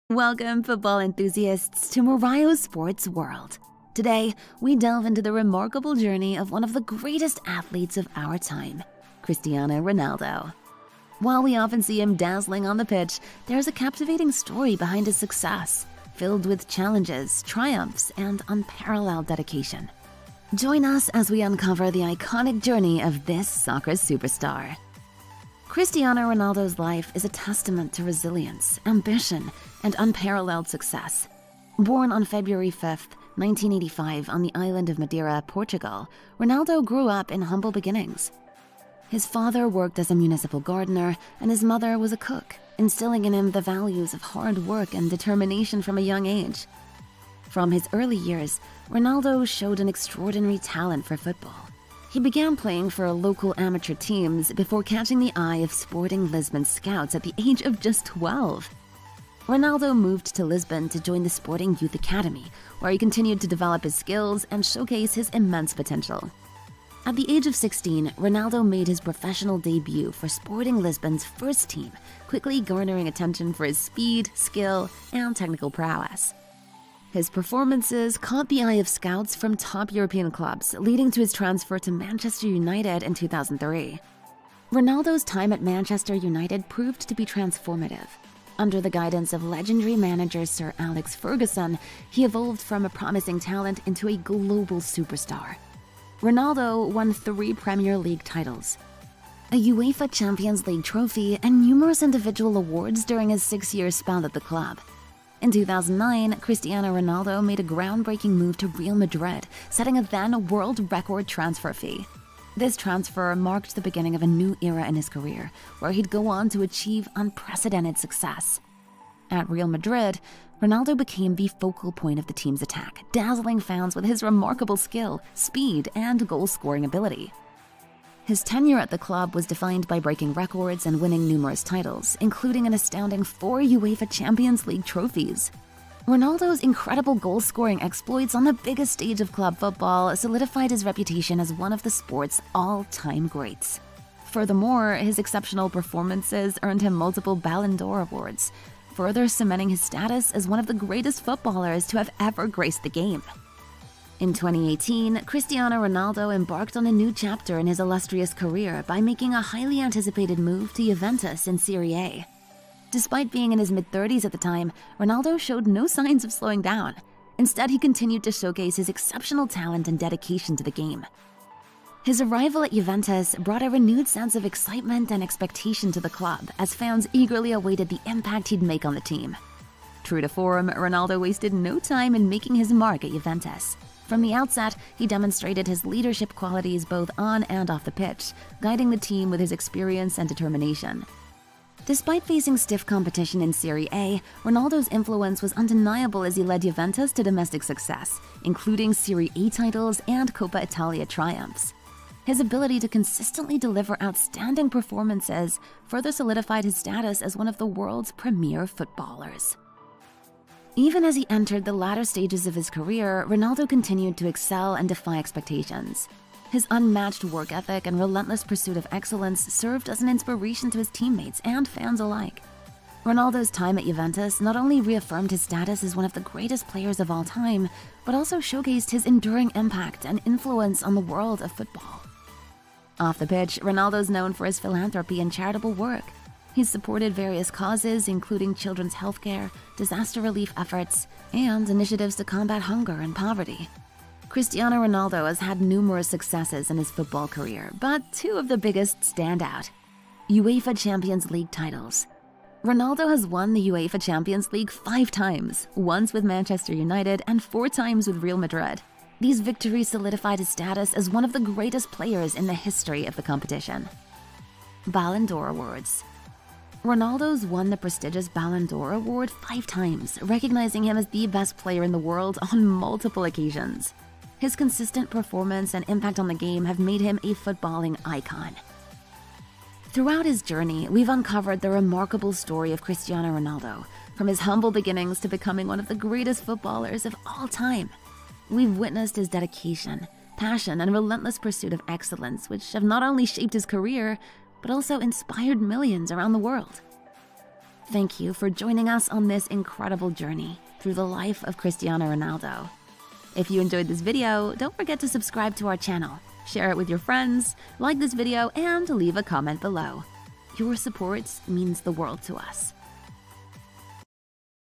Cristiano Ronaldo Mapping the Iconic Path of a Soccer Superstar
Embark on an unparalleled expedition through the storied career of Cristiano Ronaldo, the luminary whose impact on the world of football transcends borders. From his humble beginnings on the sun-kissed shores of Madeira to his ascension as a global icon, Ronaldo's journey is a testament to resilience, dedication, and unparalleled talent.

Born on February 5, 1985, in the idyllic setting of Madeira, Portugal, Ronaldo's story is one of perseverance and triumph against the odds. Join Morayo Sports World as we meticulously trace the contours of Ronaldo's iconic path, navigating through the pivotal moments that shaped his destiny.

In this captivating video narrative, we delve into the genesis of Ronaldo's footballing prowess, from the playgrounds of his youth to the prestigious academies of Sporting Lisbon, where his prodigious talent first began to blossom. Witness his meteoric rise through the ranks, culminating in a transformative spell at Manchester United, where he captured the imagination of fans worldwide with his electrifying performances.

But Ronaldo's journey is not merely a tale of individual brilliance; it's a saga of team triumphs, record-breaking transfers, and unparalleled success on the grandest stages of football. With exclusive insights, archival footage, and expert analysis, we unravel the layers of Ronaldo's illustrious career, offering a comprehensive understanding of the man behind the legend.

Prepare to be enthralled as we revisit some of Ronaldo's most iconic moments on the pitch, each a testament to his unmatched athleticism, skill, and unwavering determination to be the best. From his awe-inspiring goals to his unmatched work ethic, Ronaldo's legacy continues to inspire generations of football enthusiasts worldwide.

Whether you're a fervent supporter or a curious observer, this video promises to captivate, educate, and leave you in awe of the indelible mark Cristiano Ronaldo has left on the beautiful game. Join Morayo Sports World on this epic odyssey through the annals of football history, where the legend of Cristiano Ronaldo shines brighter than ever before.